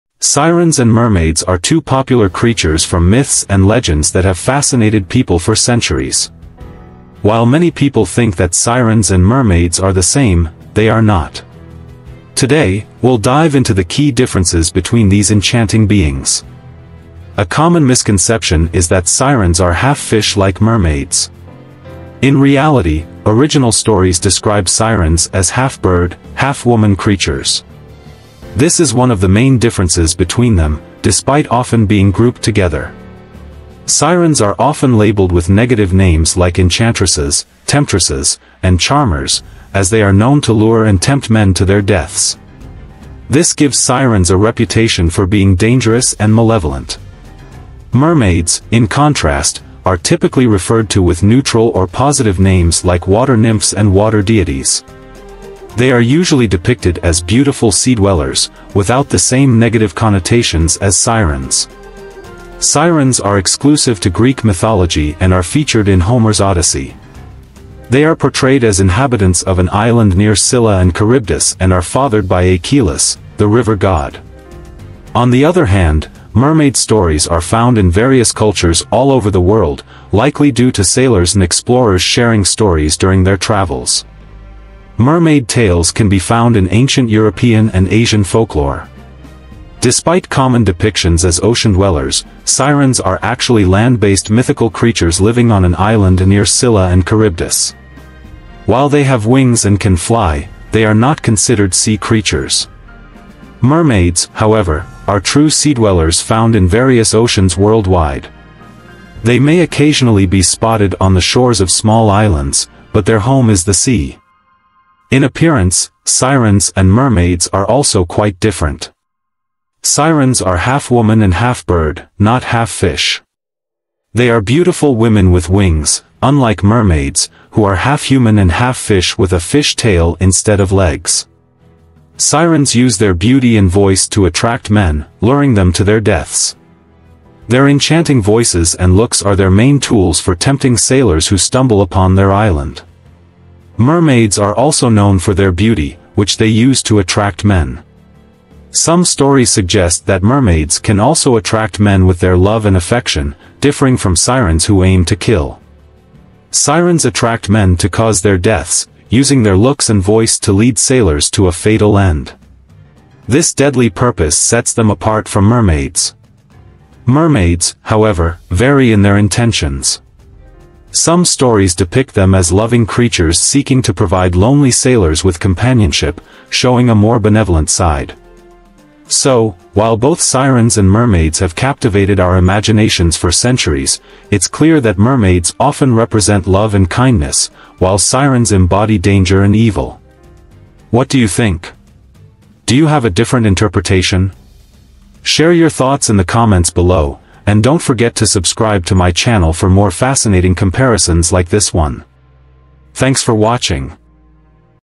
Mermaids Vs Sirens: What Sets Them Apart?
Discover the differences between mermaids and sirens in this fascinating video! Learn about their distinct features, abilities, and characteristics that set them apart from each other. From their origins in mythology to their appearances and behaviors, dive into the world of these mystical creatures and unravel the mysteries surrounding them. Watch now to uncover the secrets of mermaids and sirens!